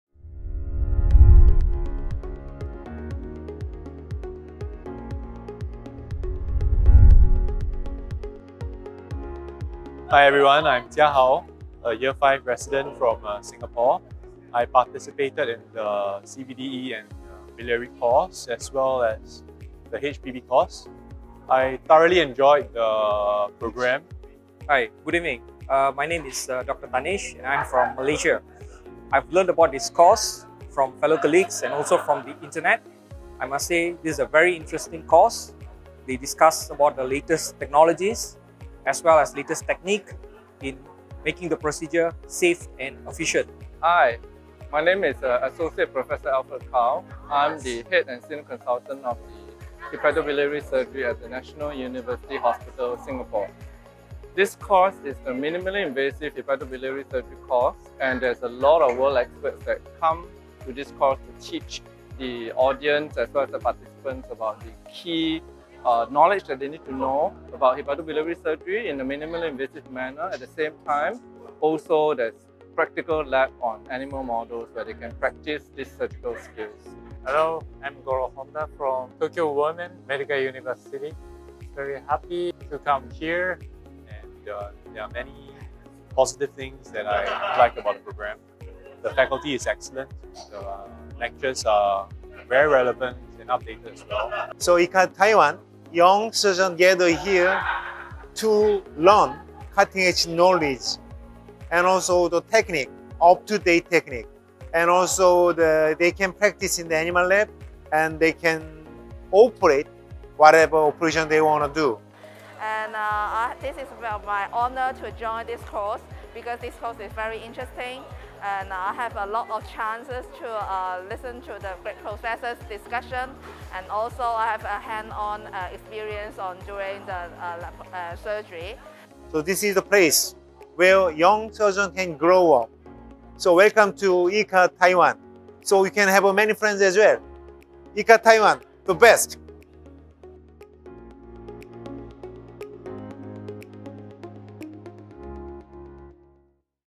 Dive into @ IRCAD-Taiwan: HEPATOBILIARY PANCREATIC COURSE | 2022
Hear what our faculty & participants have to say about our 2022 HEPATOBILIARY PANCREATIC COURSE held on December 02-04, 2022!

Register for our upcoming 2024 HEPATOBILIARY PANCREATIC COURSE in IRCAD-Taiwan!